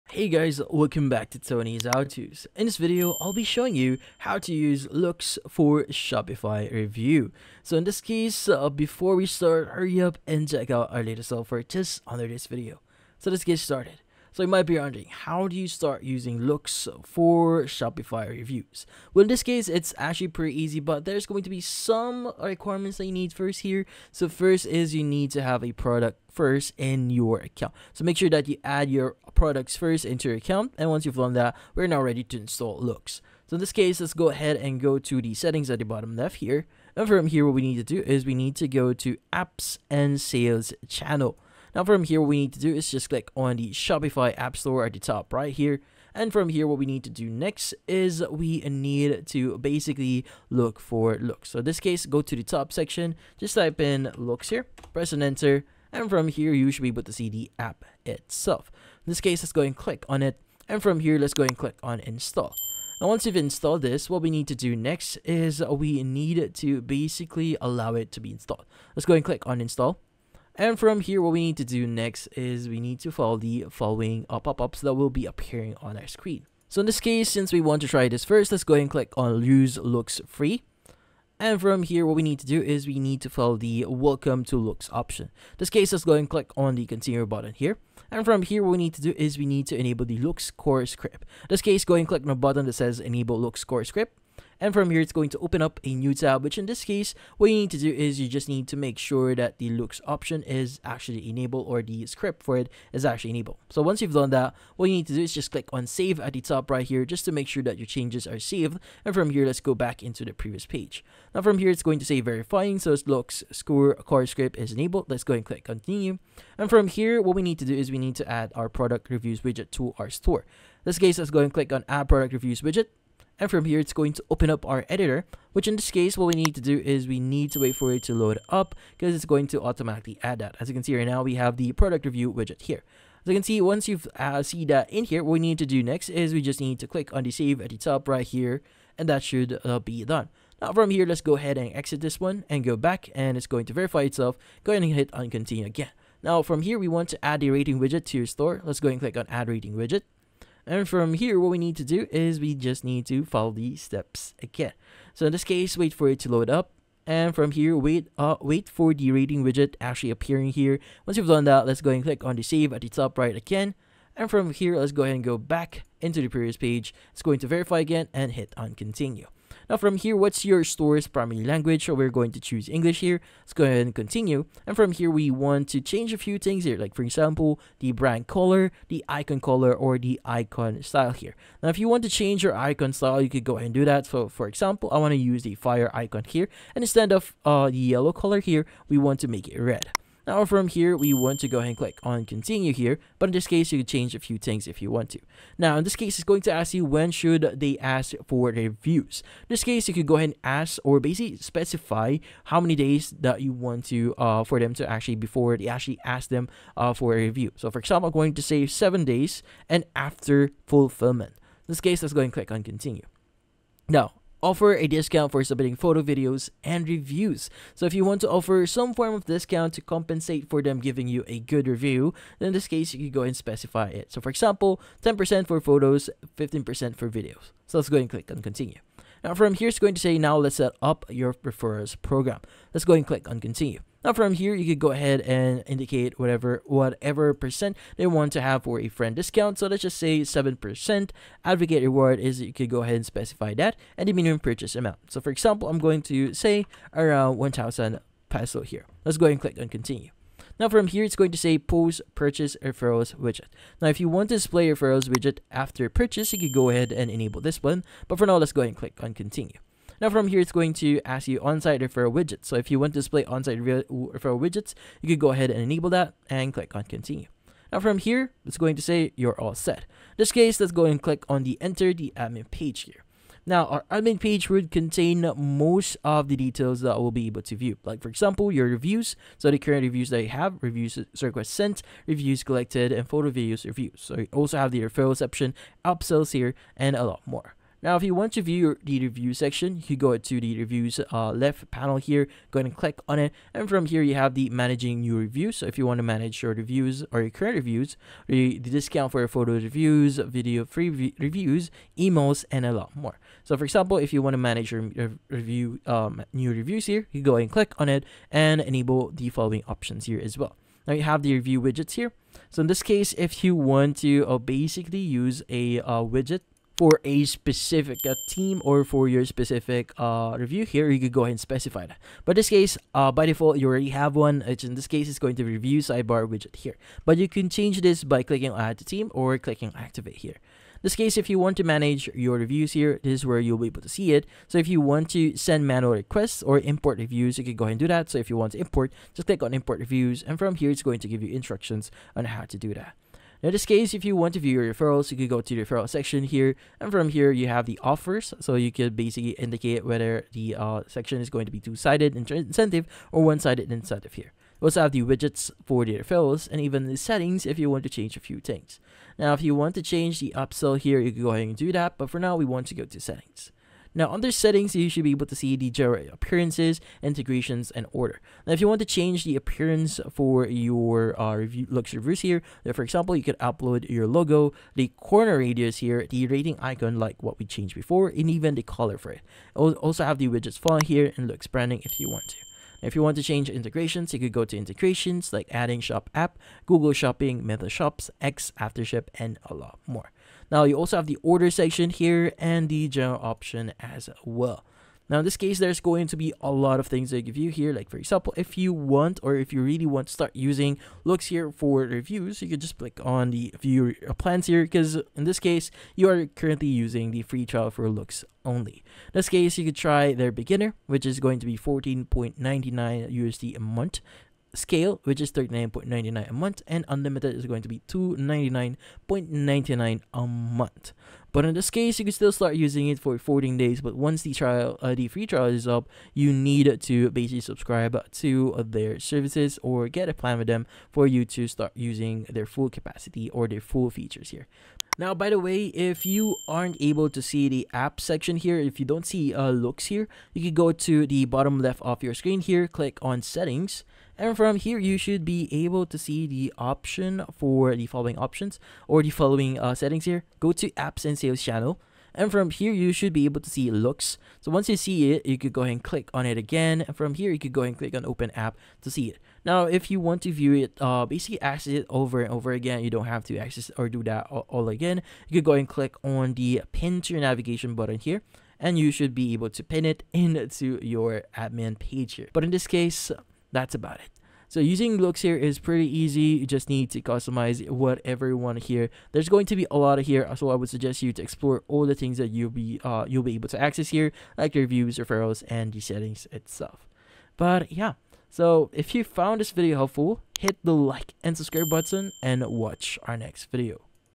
How to Use Loox For Shopify Review | Loox Reviews Shopify Tutorial (2024)
In this video, I show you How to Use Loox For Shopify Review | Loox Reviews Shopify Tutorial in 2024. The quick and easy tutorial how to use loox on shopify and add reviews to shopify store. Do you want to know step by step loox reviews shopify setup guide in 2024? Yes? Great!